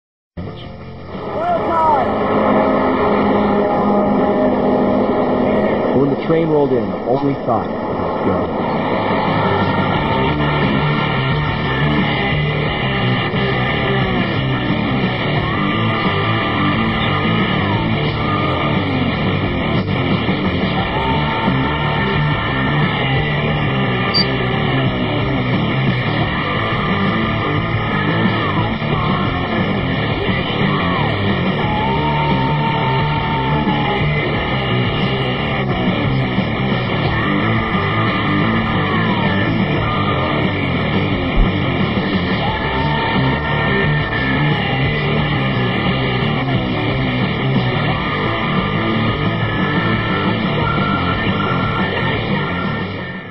Snowboarder jumps a moving train
Professional snowboarder, Temple Cummins jumps a moving train on a snowboard. Temple Cummins is totally nuts.